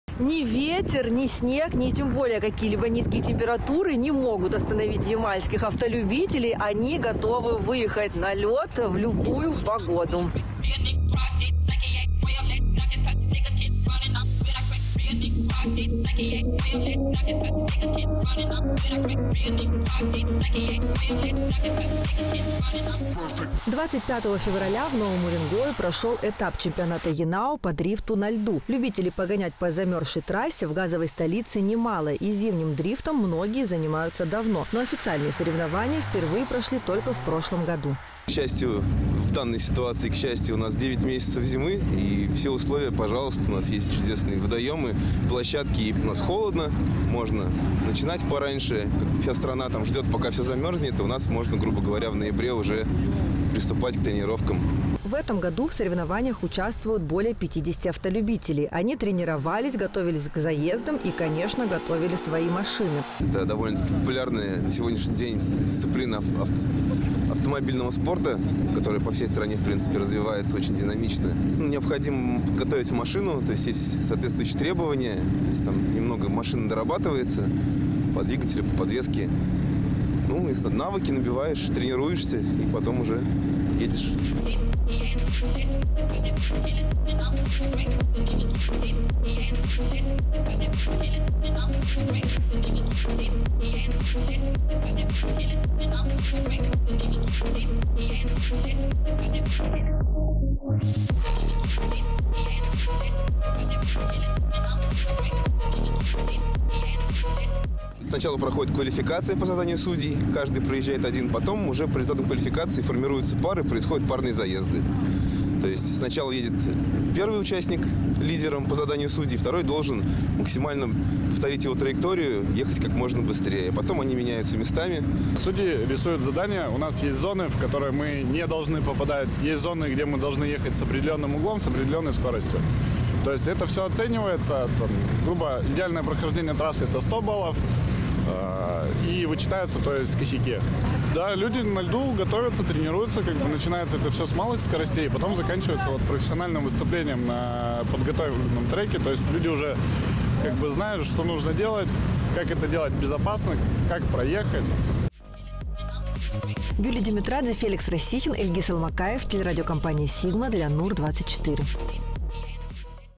Ямальский дрифт.
В Новом Уренгое прошёл этап чемпионата ЯНАО по дрифту на льду.